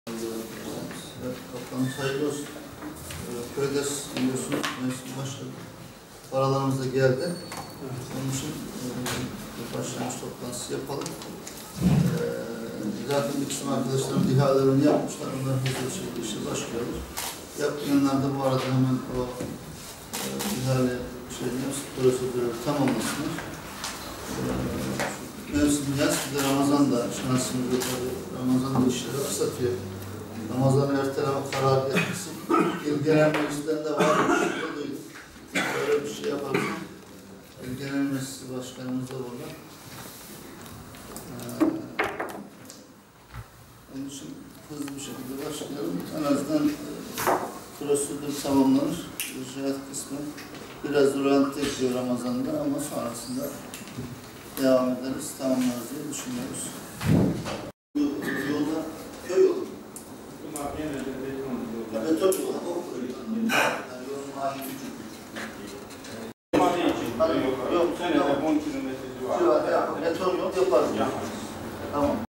KÖYDES Toplantısı "İşleri Hızlandırın"
KÖYDES İl Tahsisat Komisyonu toplantısında konuşan Sayın Valimiz Hasan Karahan işlerin hızlandırılması talimatını verdi.

Vali Karahan, "Toplantımız hayırlı olsun. KÖYDES sezonu başladı. Paralarımızda geldi. Onun için başlangıç toplantısı yapalım.  Arkadaşlarımız ihalelerini yapmışlar onları hızlı bir şekilde uygulayalım. Yapmayan arkadaşlarımızda bu arada hemen ihale prosedürlerini tamamlasınlar.  Mevsim yaz, önümüzde ramazan.  Hızlı bir şekilde işlere başlayalım."dedi.

Ardından ilçelerde yapılan çalışmalarla ilgili Kaymakamlardan tek tek bilgi alan Vali Karahan, bu yıl havaların müsait olmamasından dolayı altyapı işlerine 1 ay gibi geç başlanıldığını hatırlatarak, "Önümüzdeki süreçte Ramazan ayıda geliyor. İşlere geç başlanılmasını telefi edebilmek için çalışma tempomuzu hızlandırarak gece gündüz ve hafta sonları çalışmalarımızı sürdürmeliyiz. Programdaki işlerimizi zamanında tamamlamalıyız. İşler yapılırken de mutlak suretle denetim mekanizmasını iyi işletmeliyiz. Kısacası programdaki bütün işlerin sağlıklı olarak aksatılmadan bitirilmesini istiyorum" şeklinde konuştu.

Toplantıda 2013 yılında yapılan işlerle ilgili sunumun ardından, 2014 yılında gönderilen KÖYDES ödeneğinin ilçelere dağılımı ve projelerin durumu hakkında bilgi verildi.

2013 yılında gelen toplam  18 Milyon 352 Bin Türk Lirası ödeneğin 16 Milyon 991 Bin 433 Lirasının harcandığı belirtildi. Gelen ödeneğin 3 Milyon 570 Bin 200 Lirasının İçme Suyuna, 11 Milyon 90 Bin Lirasının köy yolları yapım ve onarımına geri kalanının ise yönetim gideri, asfalt akaryakıt boru alımı trafik işaretleri, yedek parça ve araç kiralama gibi ortak alım ödeneğine ayrıldığı, toplam 544 adet projeden 520'sinin bitirildiği, 7'sinin %70 ve üzerinin tamamlandığı, 9'unun devam ettiği, 4'ünün ihale aşamasında olduğu 4'üne ise henüz başlanmadığı belirtildi.

2014 yılında ise gelen toplam 13 Milyon 98 Bin 141 Türk Lirası ödeneğin 1 Milyon 406 Bin 631 Lirasının harcandığı, gelen ödeneğin 2 Milyon 934 Bin 236 Lirasının İçme Suyuna, 9 Milyon 476 Bin 507 Lirasının köy yolları yapım ve onarımına geri kalanının ise yönetim gideri, asfalt akaryakıt boru alımı trafik işaretleri, yedek parça ve araç kiralama gibi ortak alım ödeneğine ayrıldığı, toplam 366 adet projeden 26'sinin bitirildiği, 2'sinin %70 ve üzerinin tamamlandığı, 94'ünün devam ettiği, 42'sinin ihale aşamasında olduğu 206'sına ise henüz başlanmadığı kaydedildi.

Toplantıya, Sayın Valimiz Hasan Karahan'ın yanı sıra, Vali Yardımcısı Yüksel Çelik, 15 ilçe Kaymakamı, İl Genel Meclis Başkanı Mürşit Gürel, İl Özel İdare Genel Sekreteri Hüseyin Taşkın, İl Sağlık Müdürü Oprt. Dr. Ahmet Bal, İl Gıda Tarım ve Hayvancılık Müdürü Dr. Muhammet Angın ve diğer yetkililer katıldı.